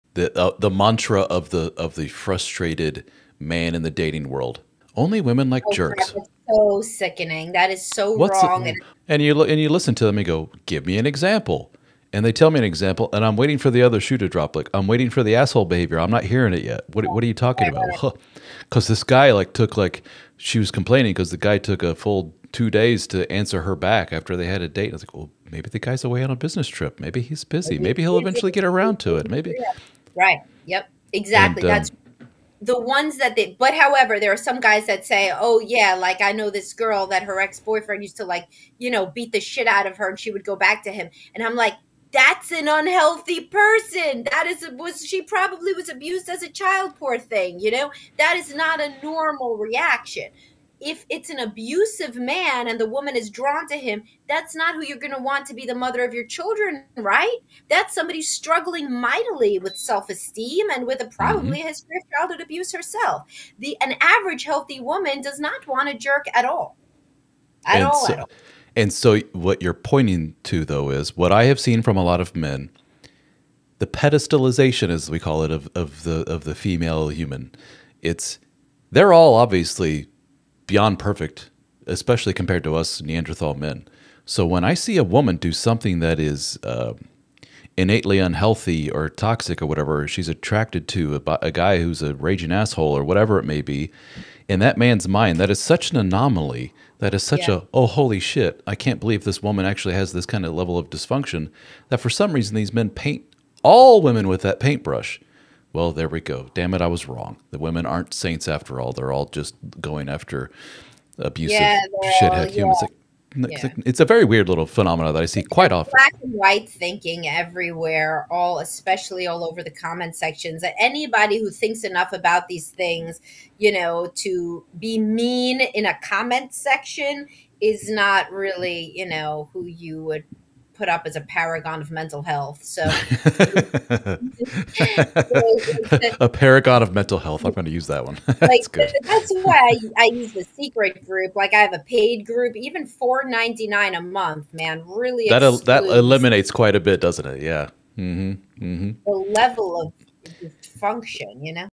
Is It True That Women Love Jerks?
Welcome to the Dad Starting Over Podcast. Today on the show, we're joined by @drpsychmom to discuss the topic of whether or not women really love jerks. This is a question that has been debated for centuries, and there is no easy answer. However, Dr. Psych Mom will provide some insights into the psychology of attraction and help us to understand why some women might be drawn to men who are perceived as jerks.
▶️ This is a clip from a live chat I had with @drpsychmom, You can view the full recording

It's a common trope: the "nice guy" who laments that women only seem to be attracted to "jerks." This notion stems from a flawed perspective that paints women with a broad brush, attributing a single trait to an entire gender.

A more nuanced understanding reveals that women, like any group of individuals, exhibit a wide range of preferences and behaviors. While some women may be drawn to certain qualities in men that could be perceived as "jerky," this doesn't mean that all women are inherently attracted to such individuals.

Many factors contribute to a woman's choice of partner, including personal experiences, emotional needs, and societal expectations. To suggest that women are solely attracted to "jerks" is a gross oversimplification that ignores the complexities of human relationships.

The notion of the "nice guy" often implies a sense of entitlement, where men feel that their kindness should automatically translate into romantic reciprocation. This belief disregards the fact that attraction is a multifaceted phenomenon that goes beyond mere niceness.

Instead of perpetuating these harmful stereotypes, it's important to acknowledge the diversity of human preferences and behaviors. Women are individuals with their own unique desires and expectations, and their choices in partners should not be generalized or labeled.